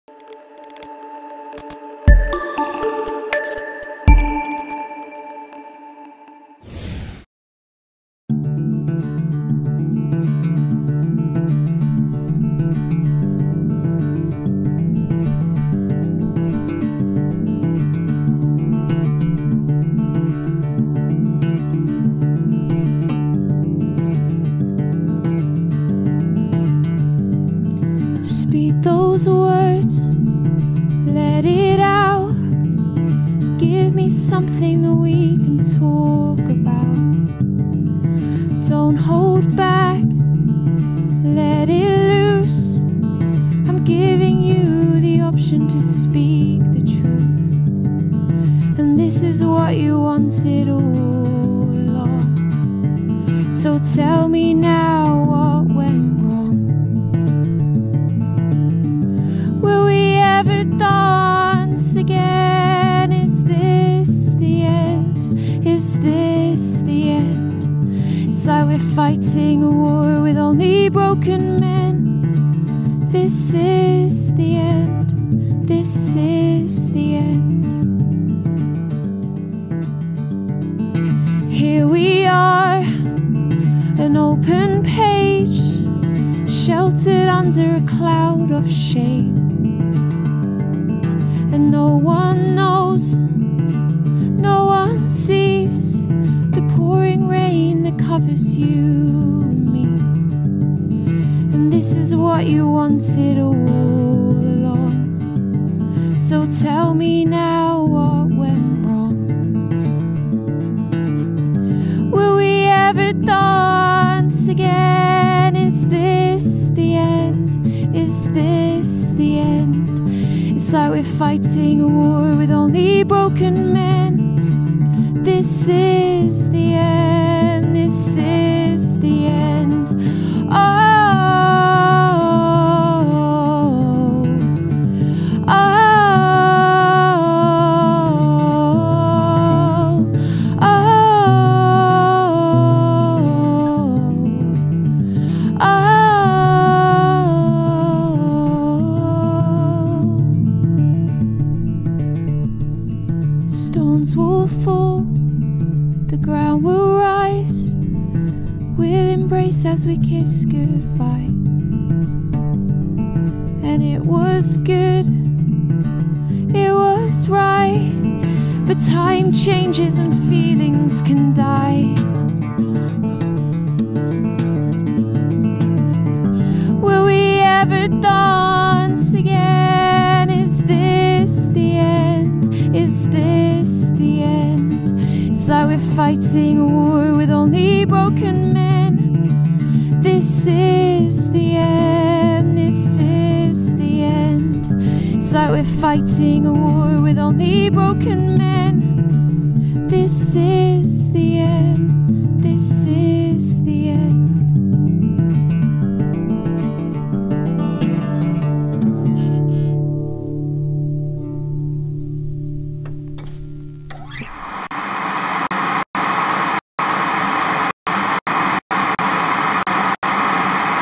Hour 13: Natasha North - The End, Seer Green.
To celebrate the summer solstice BBC Introducing hosted 24 gigs in 24 hours.